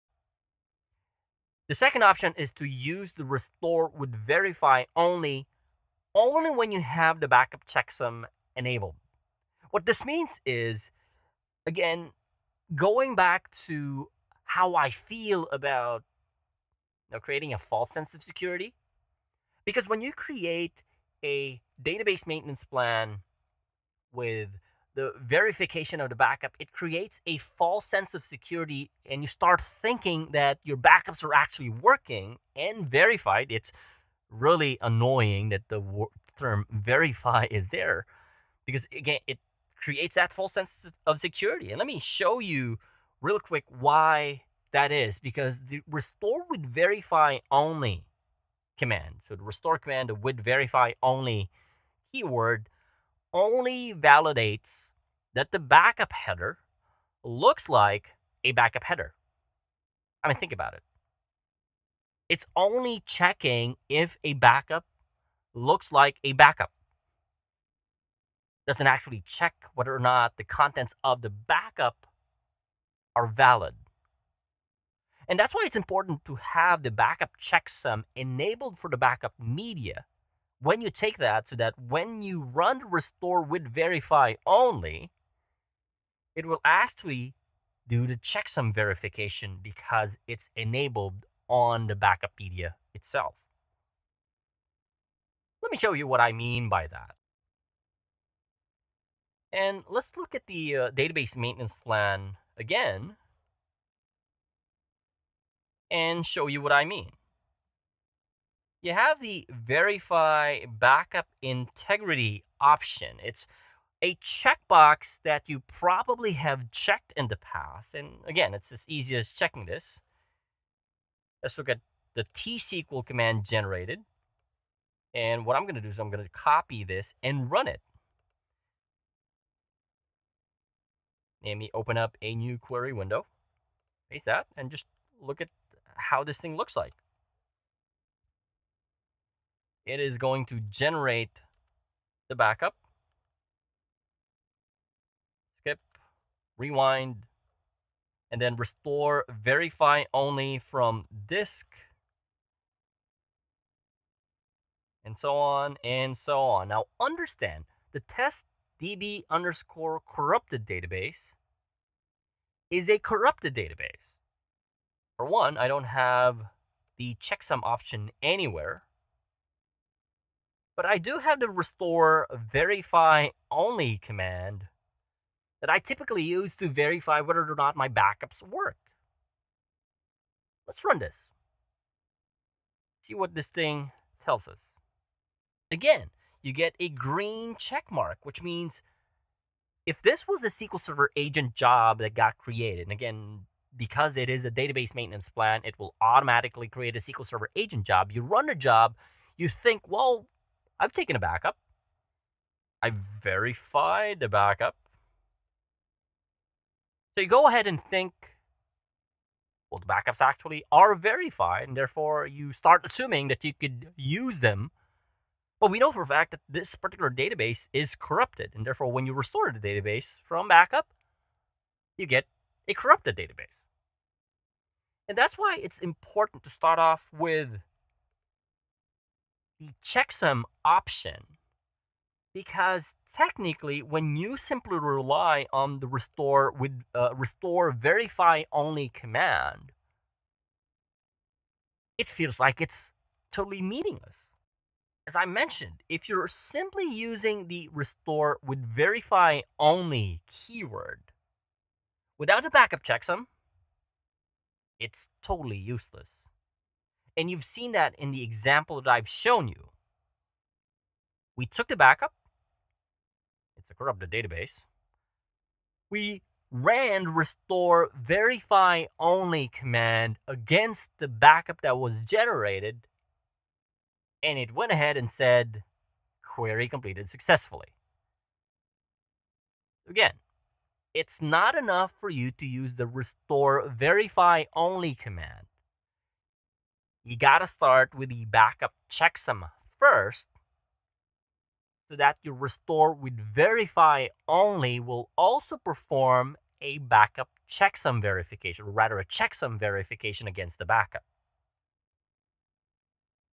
The Truth About SQL Server Backup Verification Using The GUI
This is a continuation of the video on SQL Server Backup Checksums. If you are taking backups of your SQL Server database using a database maintenance plan and using the Backup Database Task, you need to watch this video. Don't get caught unaware about this option because it probably isn't doing what you expect it to do.